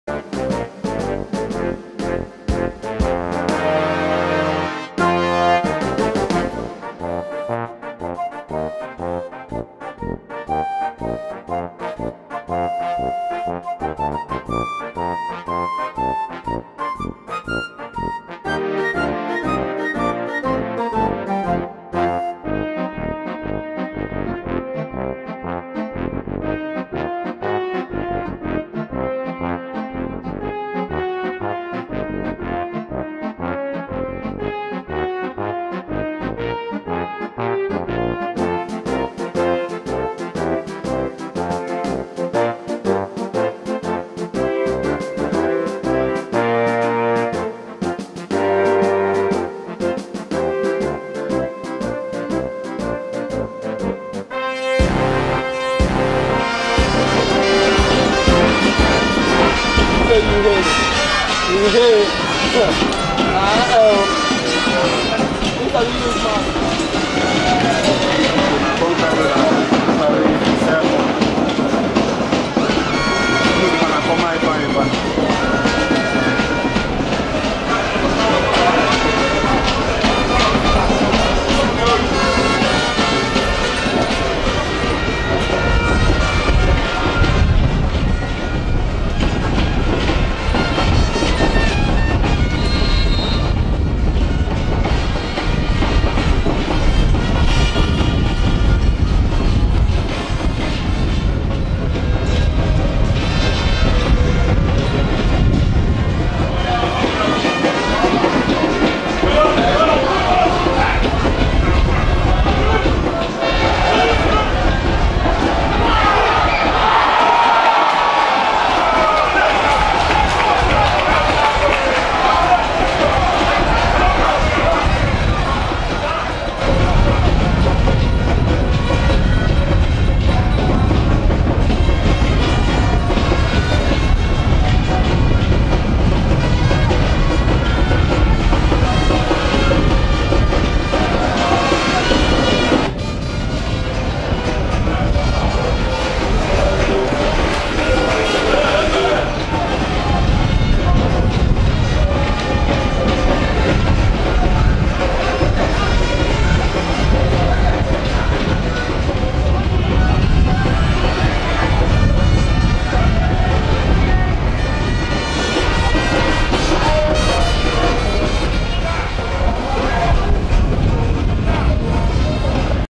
FC Ifeanyi Ubah's Ebuka Nwenyi in Action
Highlighted Videos of the prospective Left Back with a great Future